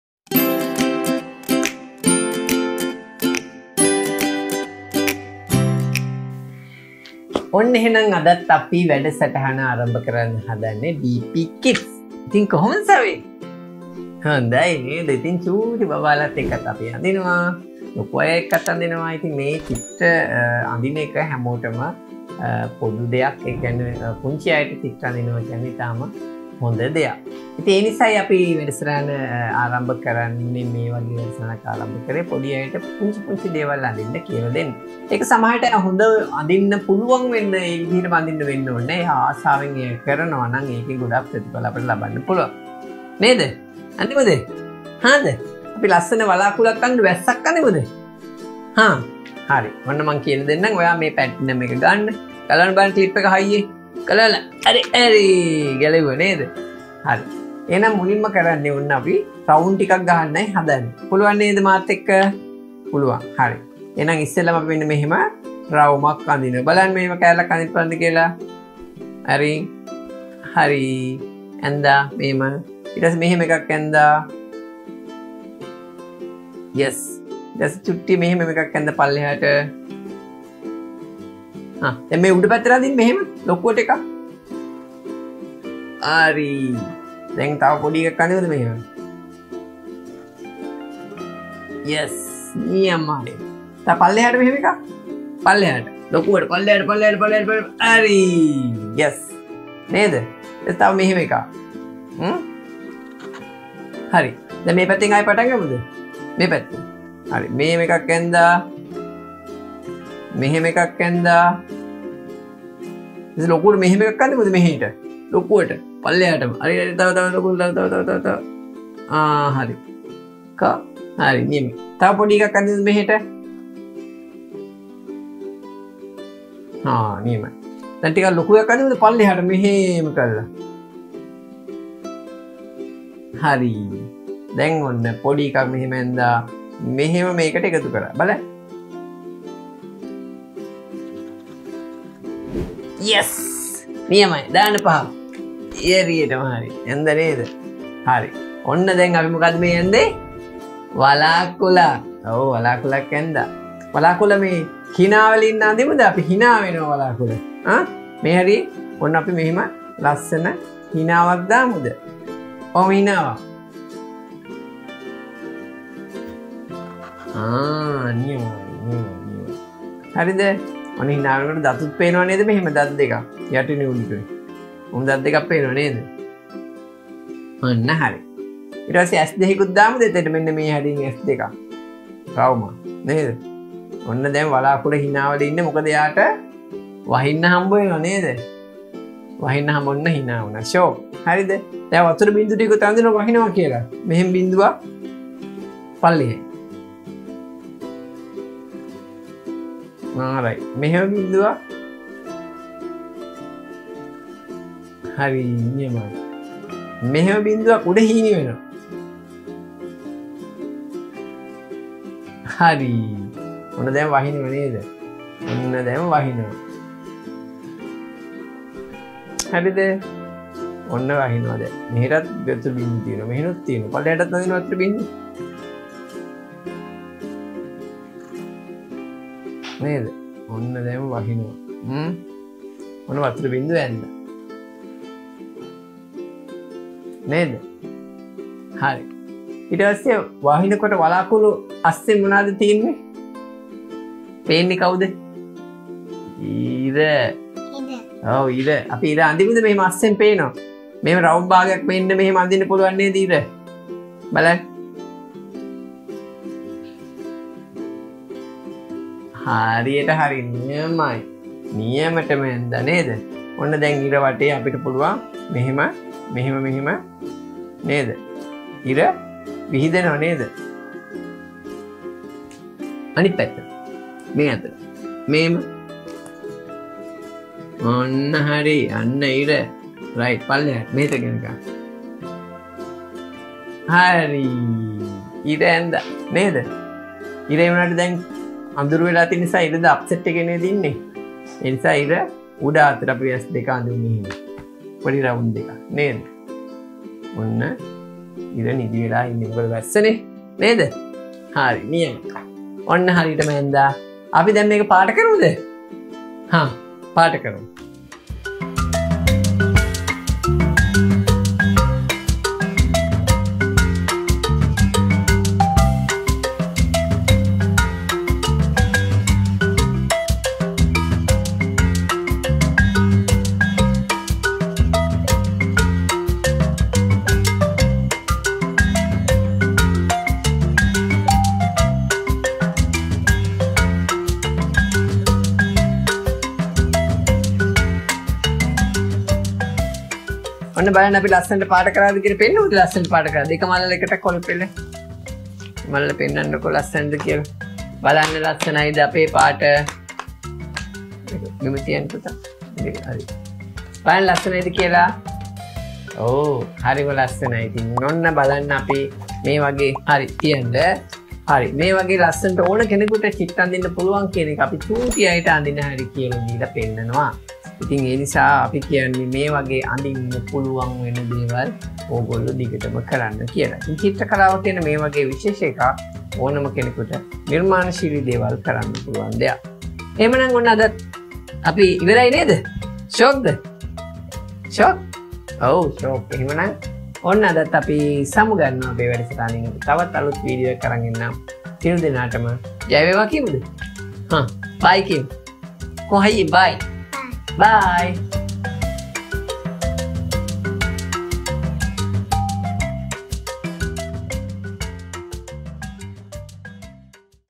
වැහි වළාකුලක් අඳිමු | Let's draw a Rain cloud - DP KIDS ARTS & CRAFTS (Video 296)
▶️ මෙම විඩියොව තුලින් වැහි වළාකුලක් චිත්‍රයට නගන්නේ කොහොමද කියලා බලමු.
▶️ In this video we will see how to draw a Rain cloud.

TIMESTAMPS:
Introduction - 0:00​​
Let's draw a Rain cloud - 1:12